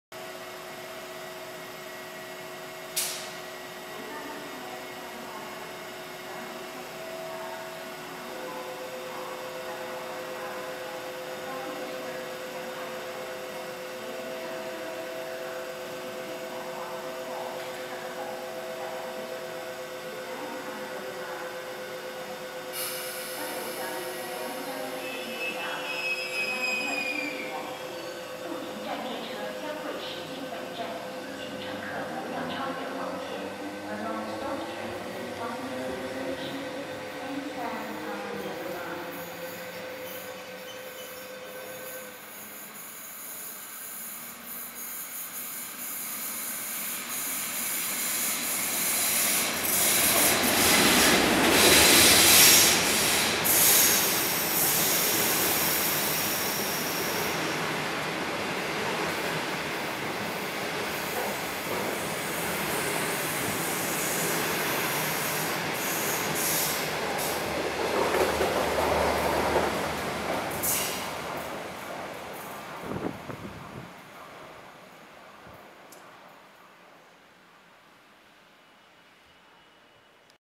Southbound Through Train (GuangZhou to Kowloon) T819
The Southbound Through Train T819 (the new Timetable from 1 July 2007) DF11 0017 + 25Z in the night time.

Time: 6:39pm
Date：28/02/2008
Canon G7 30 fps 640x480 standard
Place: Mong Kok Station
Software: Apple QuickTime Pro 7.2.0 (AVI to MOV)